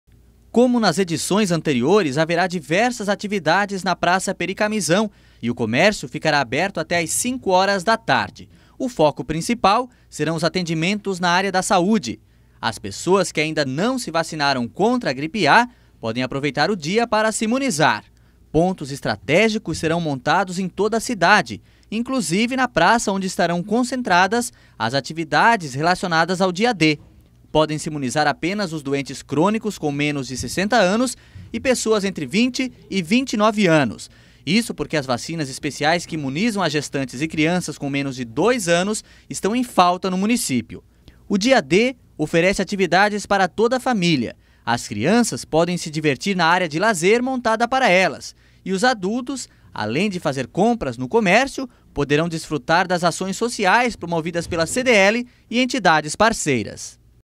Dia D lembrará o Dia Mundial da Saúde
Neste sábado (10/04) Tubarão terá mais um dia D. Esta edição será alusiva ao Dia Mundial da Saúde